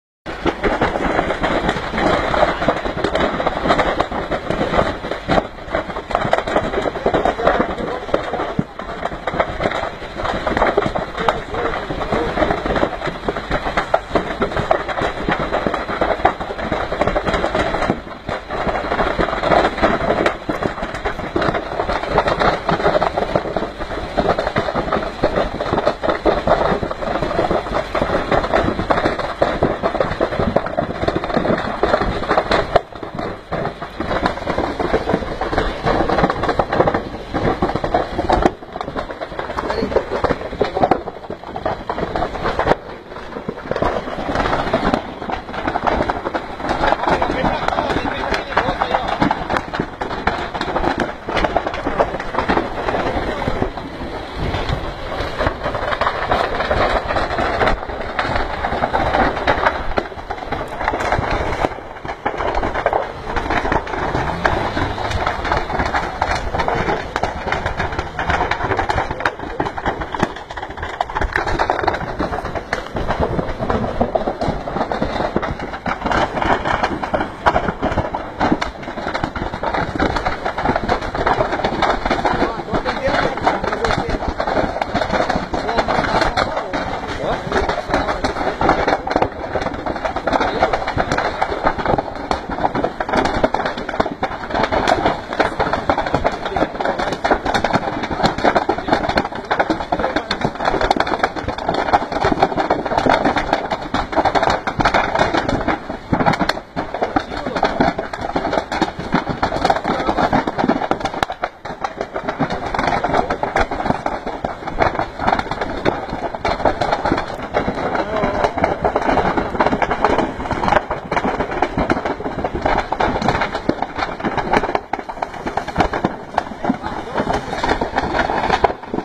Caminhão carregado de fogos explode
Video que me enviaram pelo watsap, não sei detalhes, caminhão carregado de fogos de artificios pega fogo.. Diário de bordo de um caminhoneiro